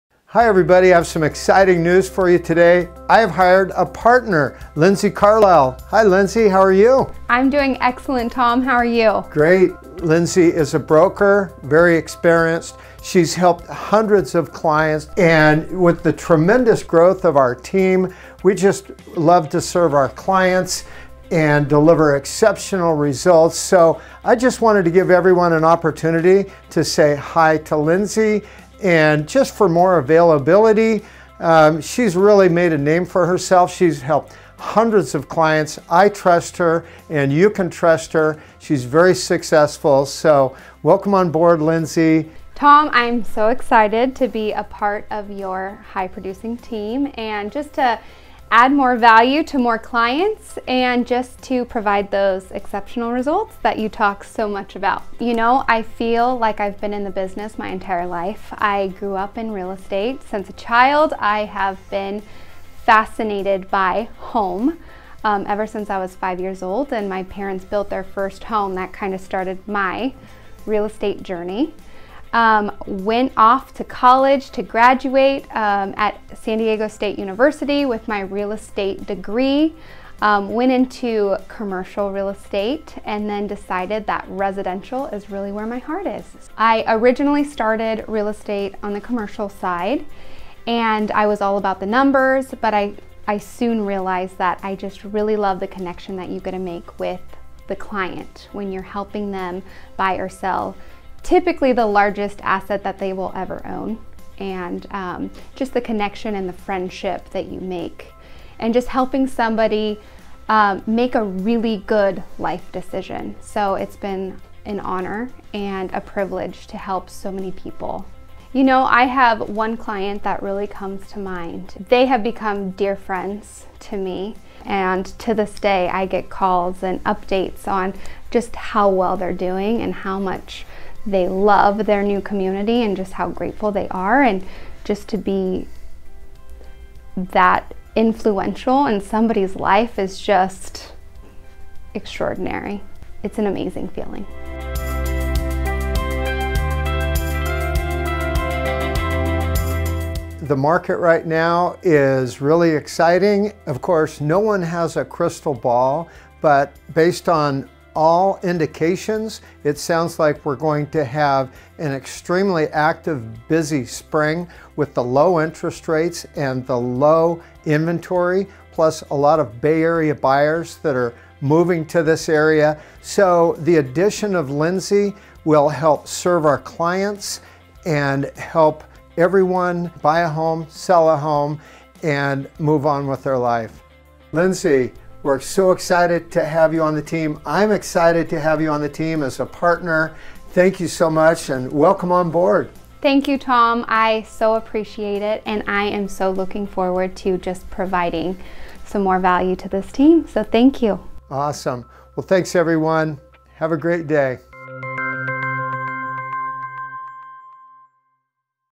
Q: Have You Met My New Rockstar Partner?
Today I’m coming to you with some truly exciting news: I’ve hired a partner, the talented Lindsay Carlile. Lindsay is a highly experienced broker who has already helped hundreds of clients, so bringing her aboard was an absolute no-brainer; we’re driven to continuously grow in such a way that allows us to better serve the diverse real estate needs of our clients. Lindsay is a proven professional who has the full confidence and backing of the Tom Daves Team—we can’t wait for you to meet her and see for yourself why she can be a trusted resource! For a formal introduction and a chance to hear a bit about the type of market that awaits us in 2021, watch this short video.

Tom Daves
Tom Daves Team
Sacramento, California
CalBRE #00581837